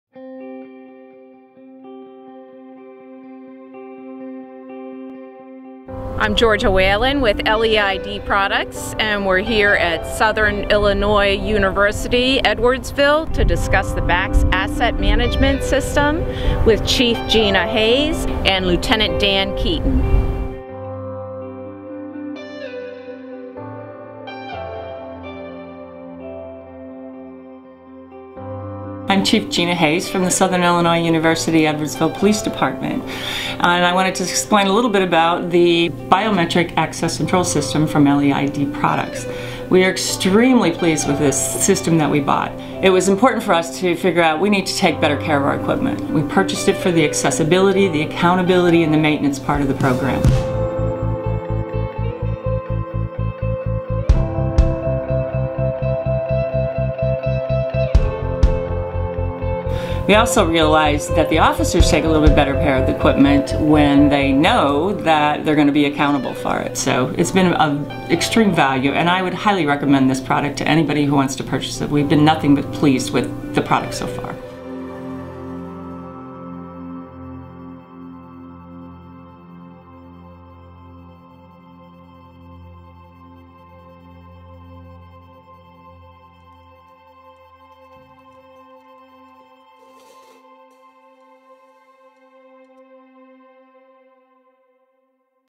Campus Safety - University Police Department Installs LEID Products BACS System to Secure Assets
Campus Safety - Southern Illinois University Edwardsville (SIUE) achieves immediate benefits using BACS asset management system for accurate control and tracking of officers shared equipment. SIUE Chief of Police Gina Hays shares her experience with LEID Products.  With 39 police officers staffing two SIUE campuses, the majority of department assets are shared. SIUE purchased the BACS Asset Management System for its accessibility, accountability and efficiency. BACS (Biometric Access Control System) requires fingerprint ID to access equipment and weapons tagged with RFID chips for tracking. Items are stored in electronic lockers. All transactions are logged - administrator can generate reports and see in real time which officer has what equipment.